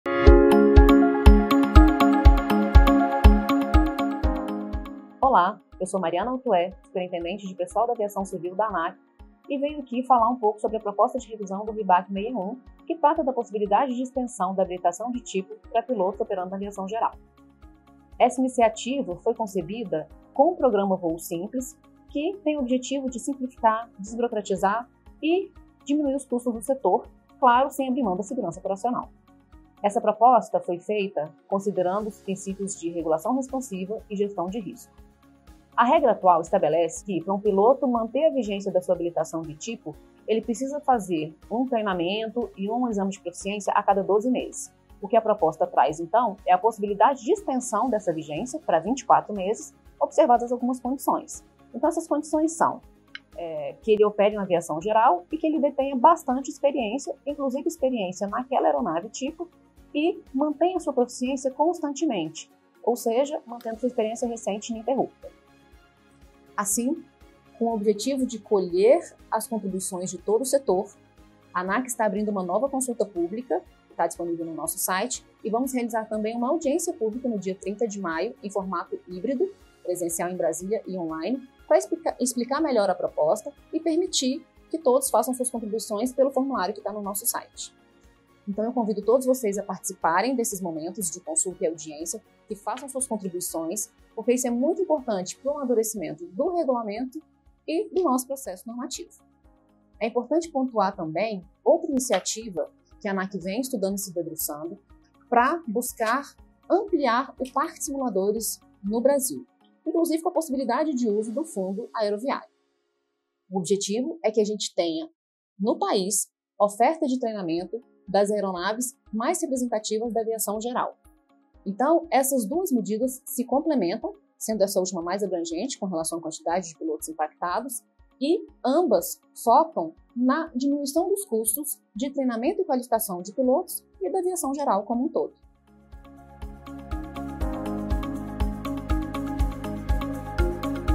RBAC 61 - Habilitação de tipo
Emenda ao RBAC 61 é submetida a nova Consulta e Audiência Pública. A proposta prevê extensão de 12 para 24 meses a vigência de treinamento em simulador de voo.